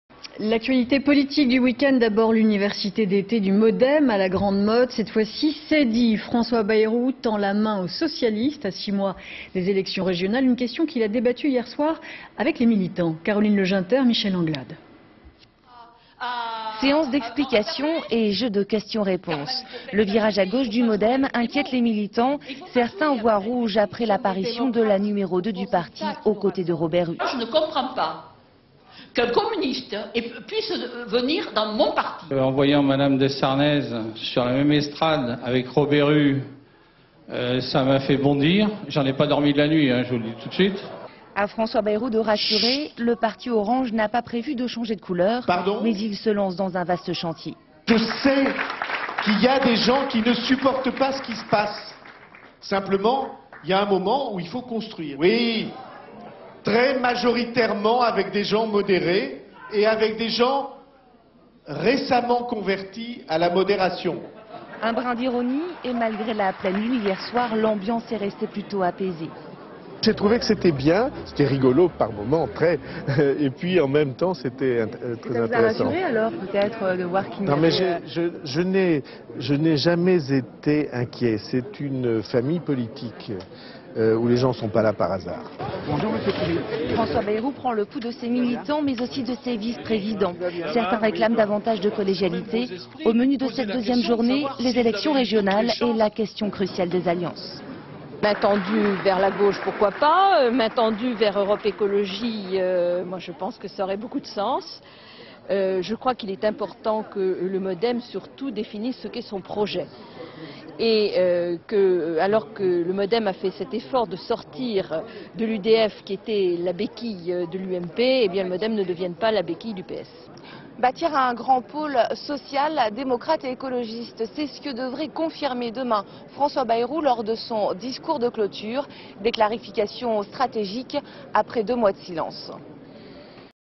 François Bayrou confirme l'alliance à gauche - Archive INA
5 septembre 2009
[Source : documentation France 3]     L'université d'été du Modem s'est ouvert hier soir à la Grande Motte. Son président François Bayrou a confirmé son désir d'alliance avec la gauche pour les élections régionales, malgré le scepticisme et la peur de certains militants.            François Bayrou :      "je sais qu'il y a des gens qui ne supportent pas ce qui se passe, simplement il y a un moment où il faut construire, très majoritairement avec des gens modérés et avec des gens récemment convertis à la modération"      A propos de son échange avec les militants "J'ai trouvé que c'était bien, rigolo par moment et en même temps très intéressant. je n'ai jamais été inquiet, c'est une famille politique où les gens ne sont pas là par hasard"            Corinne Lepage "main tendue vers la gauche pourquoi pas, main tendue vers Europe Ecologie je pense que ça aurait beaucoup de sens, je crois que ce qui important avant tout c'est que le Modem définisse son projet, alors que le MOdem a fait cet effort de sortir de l'UDF qui était la béquille de l'UMP, que le Modem ne devienne pas la béquille du PS"      PS Caroline Le Junter politique; archive television; archive tv; ina; inna; Institut National de l'Audiovisuel; french tv Images d'archive INA